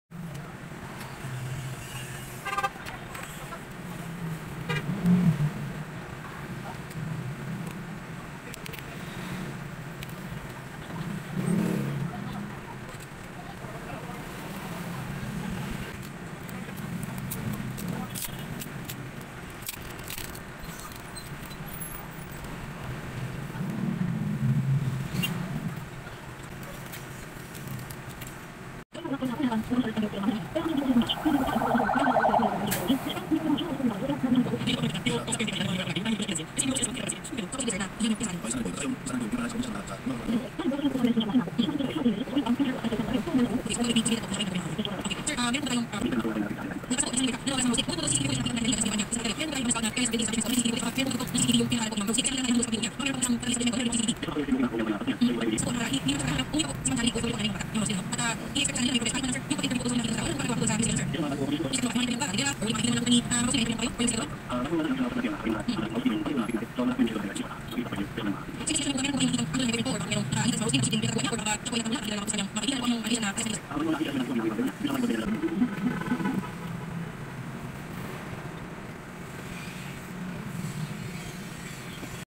Realme C2 Disassembly and Changed Lcd
Here beside Enjoy Shopping Center Consolacion Cebu @ Ace Gadget Solution